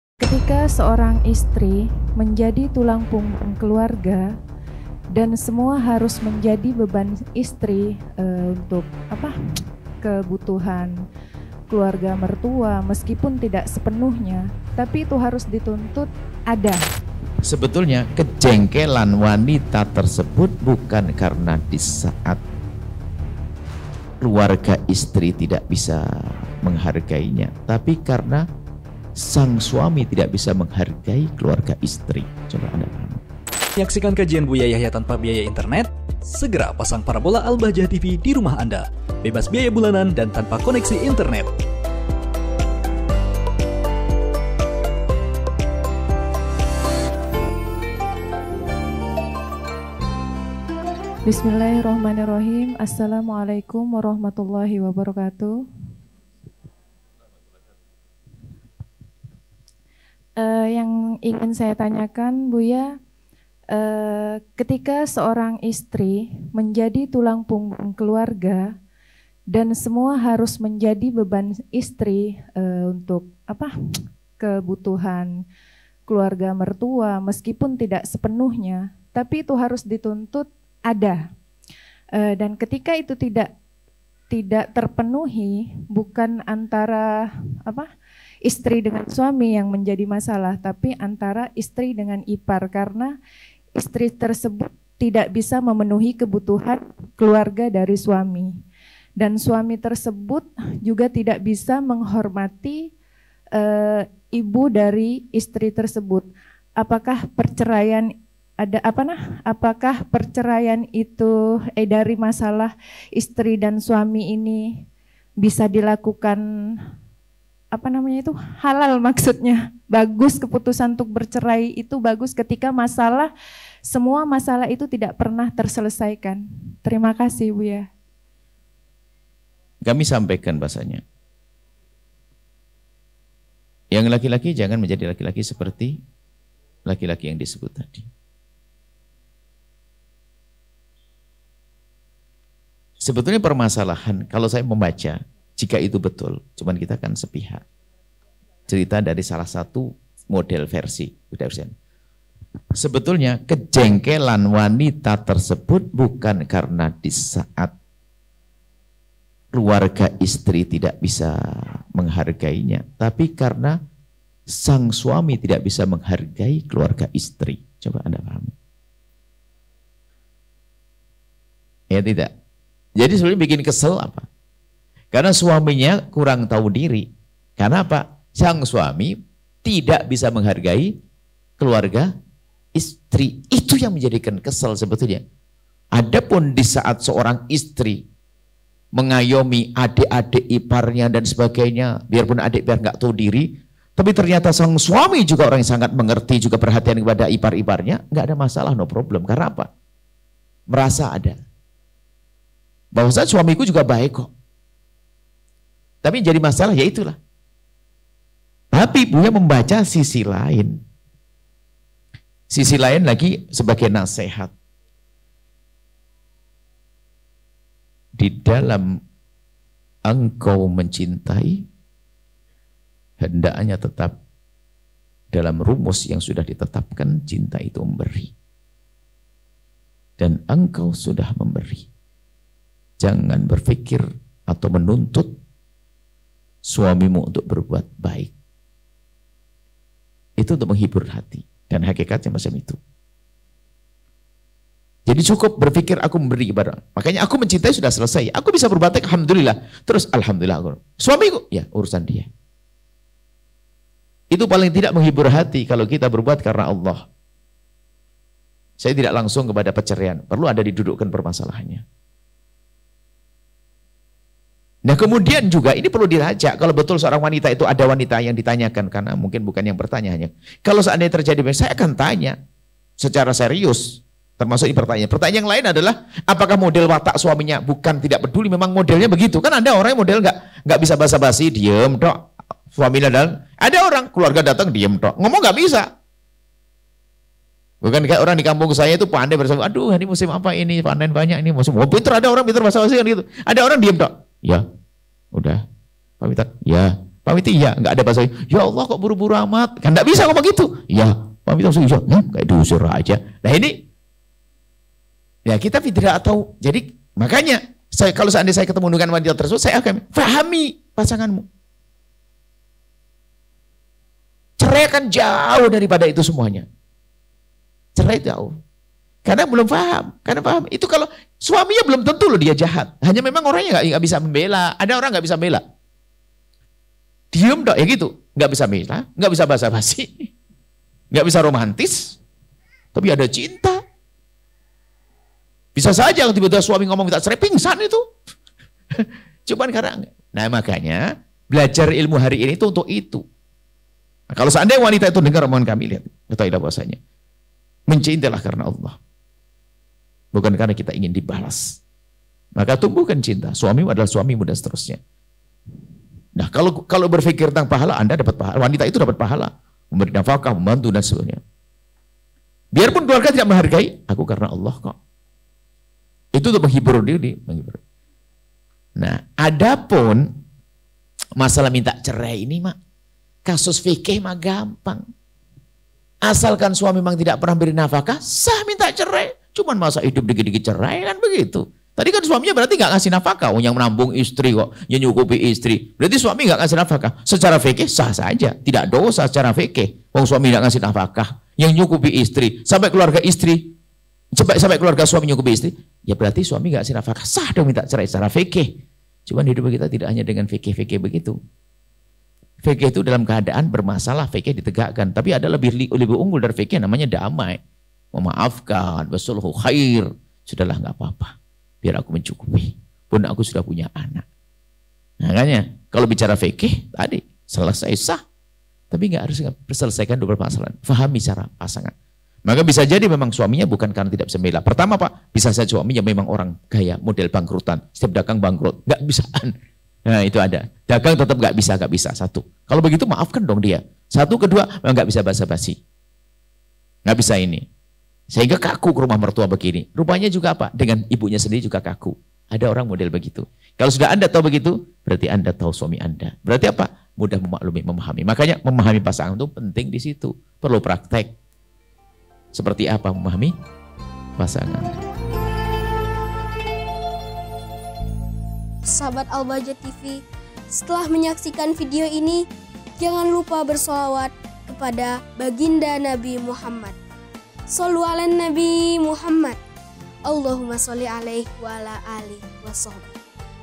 Istri menjadi Tulang Punggung Keluarga tetapi Suami tidak Tahu Diri. Haruskah Bercerai? | Buya Yahya
No. Rek : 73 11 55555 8
Kode Bank : 451
a/n : Al Bahjah TV